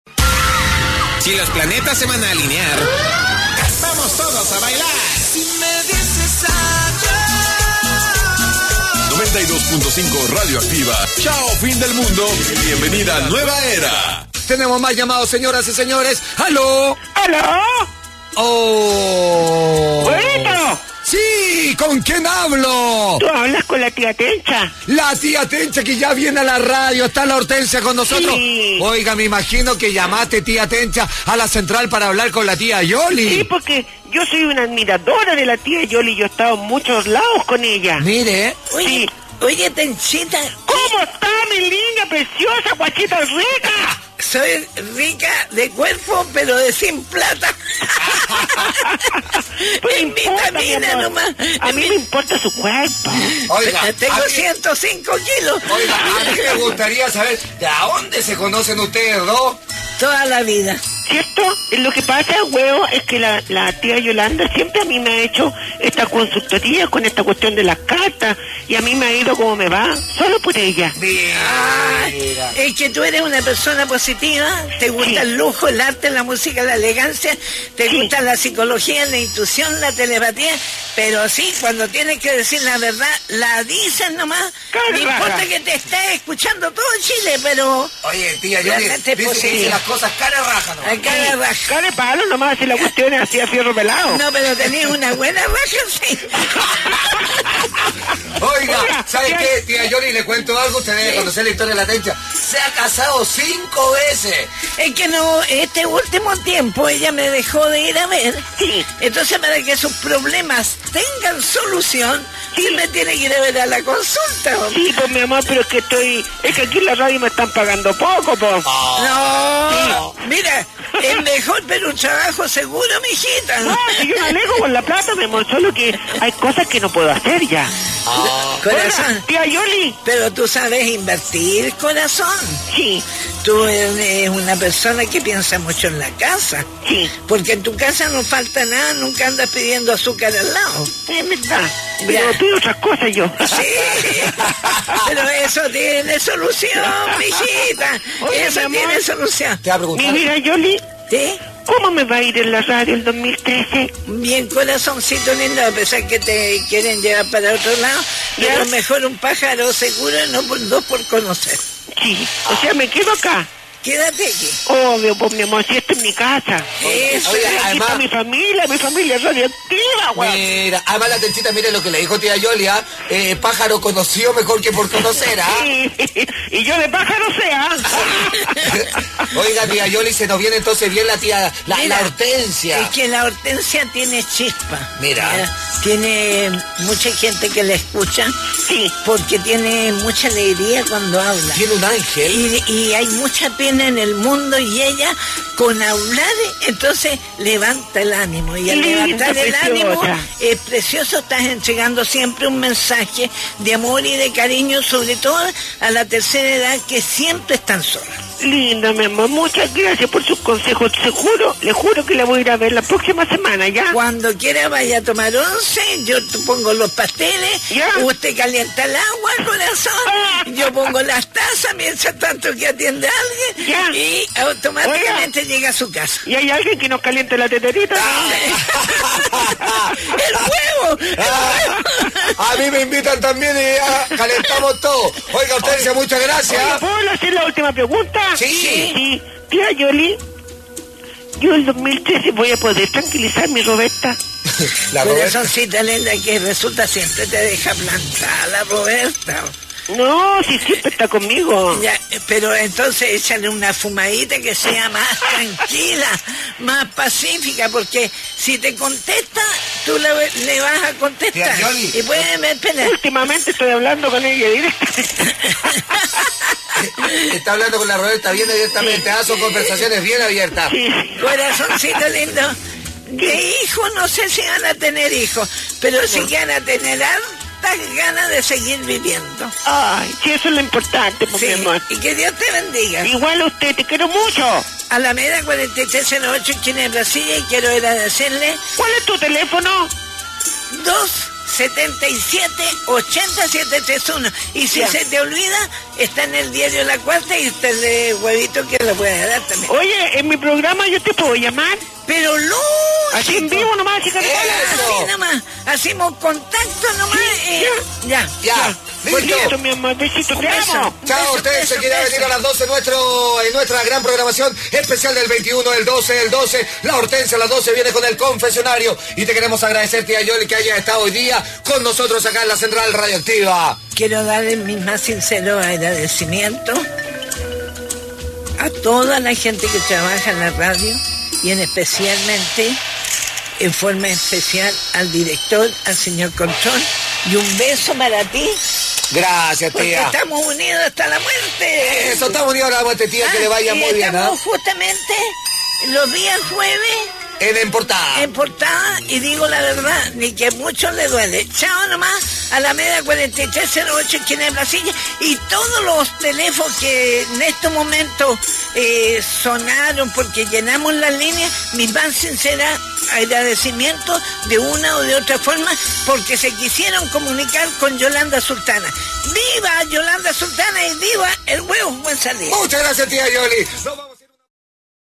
La Tencha habló con Yolanda Sultana
En nuestro Especial del fin del mundo, Yolanda Sultana habló con nuestros auditores  y hasta con la Tencha!